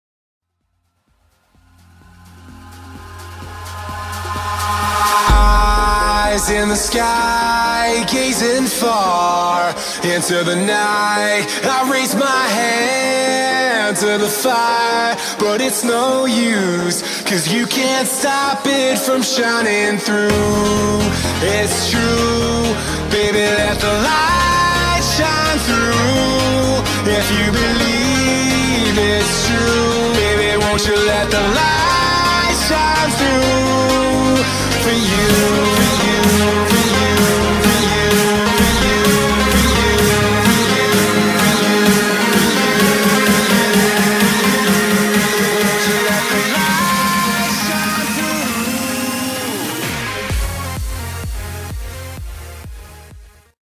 DJI Phantom drone - Switzerland - Belalp/Aletschgletscher from above
DJI Phantom 3 Flying in Switzerland, Belalp/Aletschgletscher from above, Phantom Dji, Switzerland, Schweiz, Drone flying, frome the air

|Beschreibung| von SwissAction

Flying in Belalp&Aletsch glacier

Equipment:
-Phantom 3 Advanced (1080p/60FPS)
-GoPro H4 Silver
-Magix deluxe premium 2015